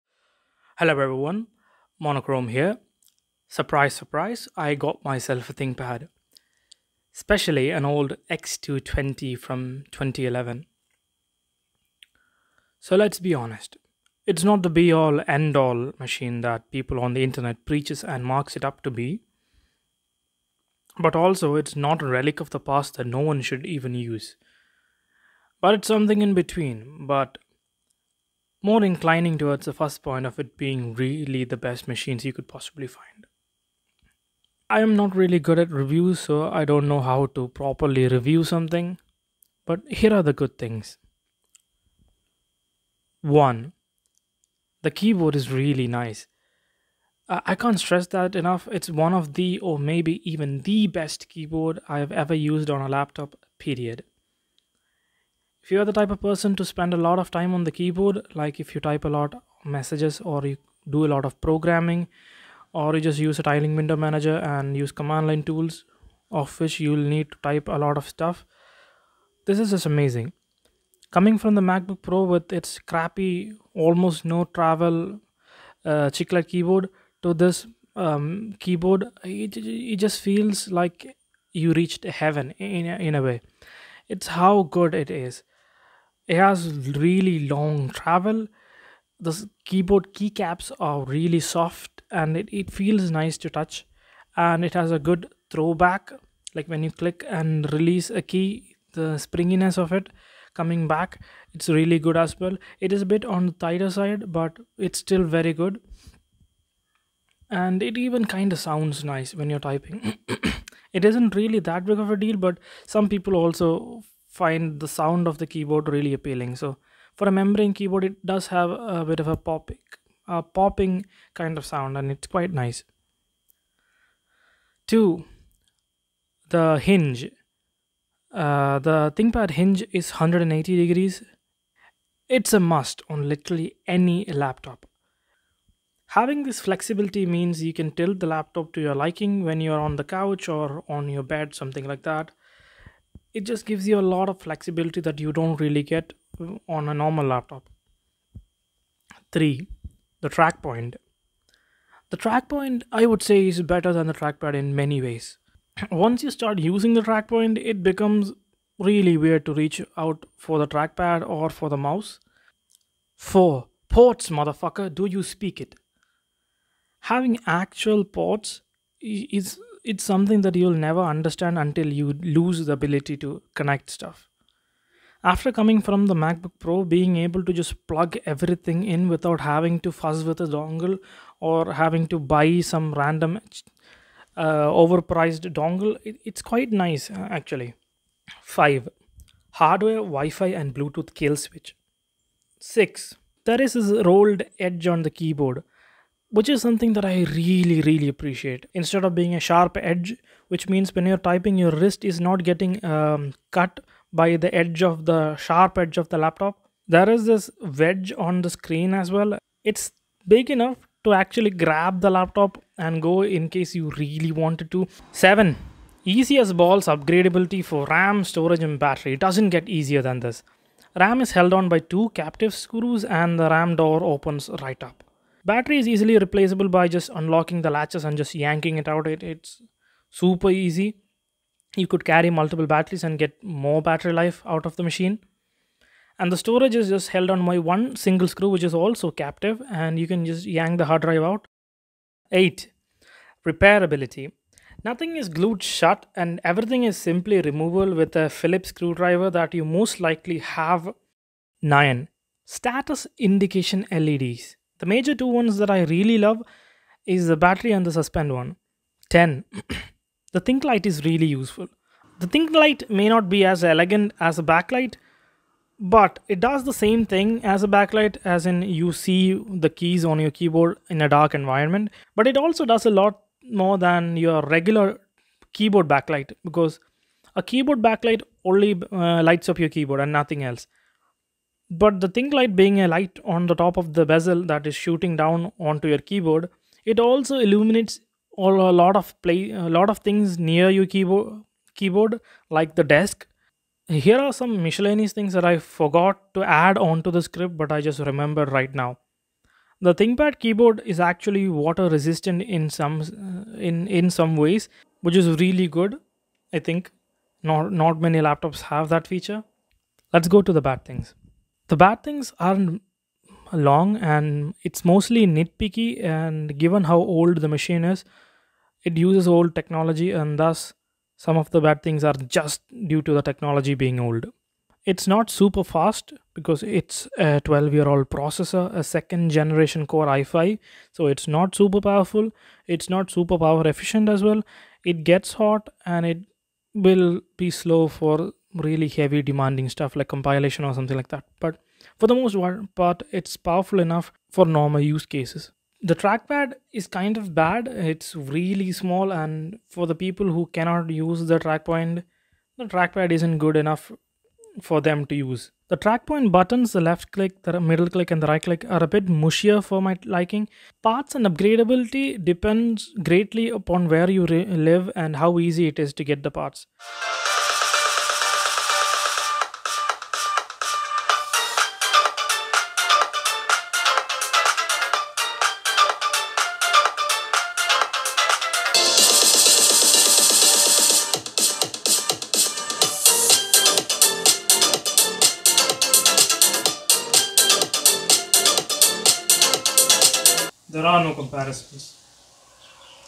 ThinkPad X220 Review (?) 2023
So, I bought am old ThinkPad for the memes and for muh freedumbs, and here's what I feel about it.